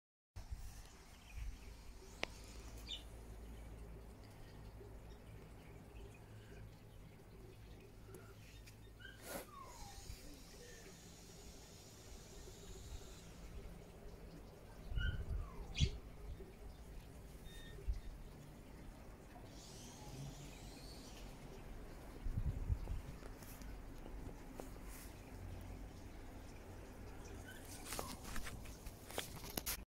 Slow worm
I found a cute little slow worm in my garden today :)